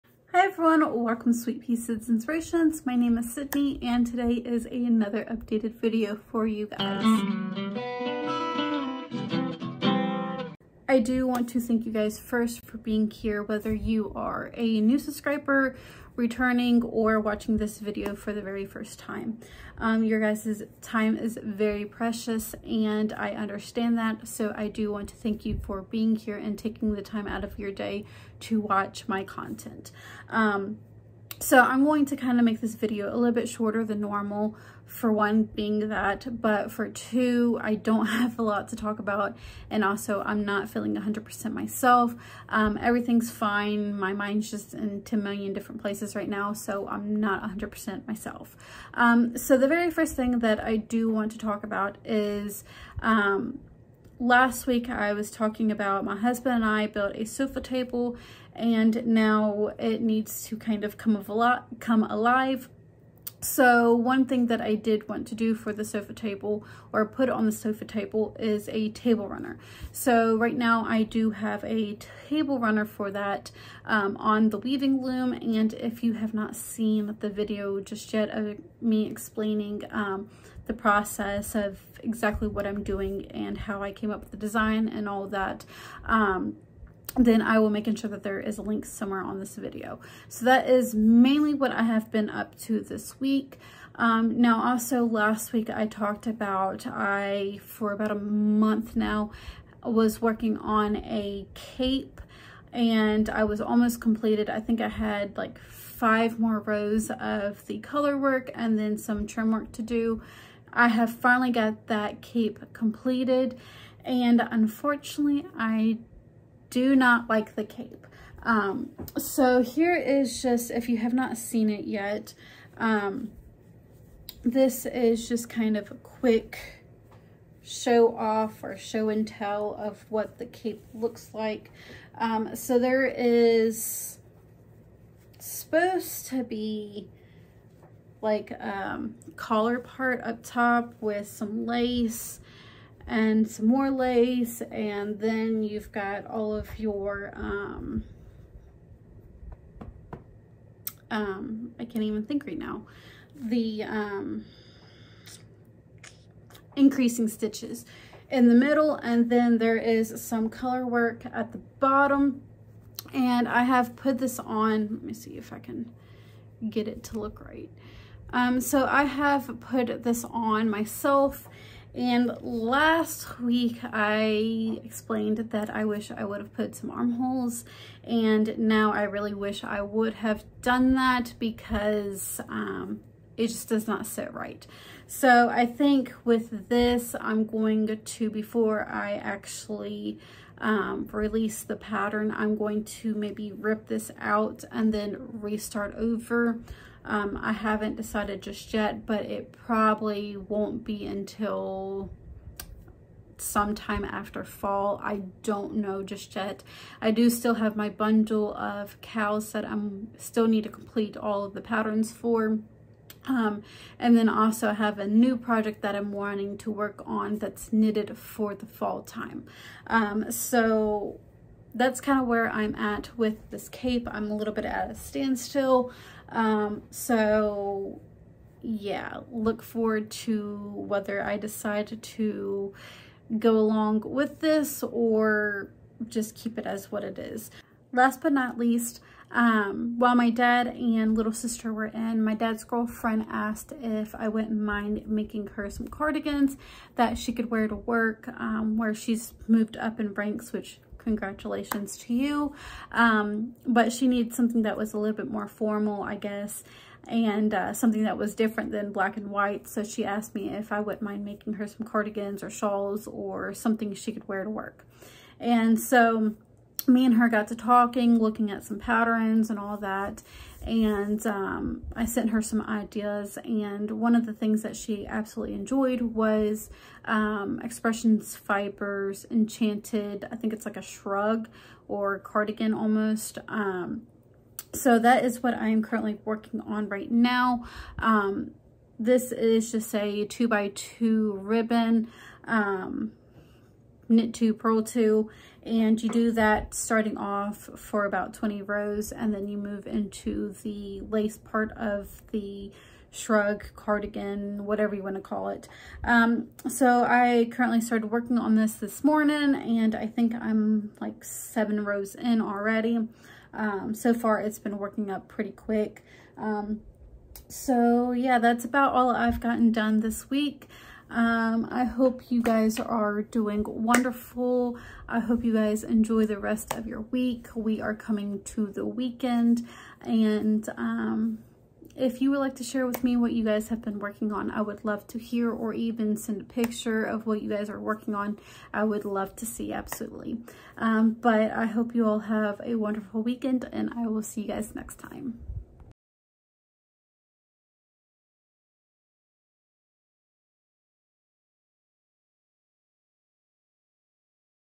Update #41 || Weaving, Knitting and more Knitting!!
Welcome to Sweetpea Cyd's Inspirations. Today just going over some Work's in Progress along with what we’ve been up to this past several weeks. Have a few new things in the works as I am switching to written patterns mainly.

Do need your help on new content ideas! or do you like the tutorials?! please let me know down in the comments!

Was not Able to work some on my Lisa Conleys Locking Filet Blanket.

All Proceeds will go to supporting the channel. Including giveaways, shipping/handing, etc.

Thank you!💛yarn